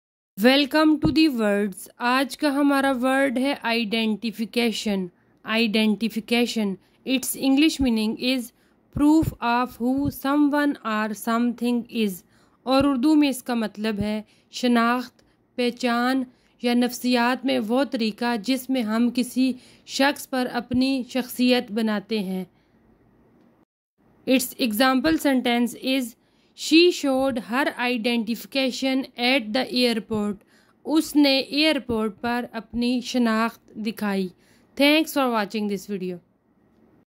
Identification meaning in urdu/hindi |How to pronounce Identification |Identification in sentence
what is Identification
Identification in urdu
english to Words
Words meanings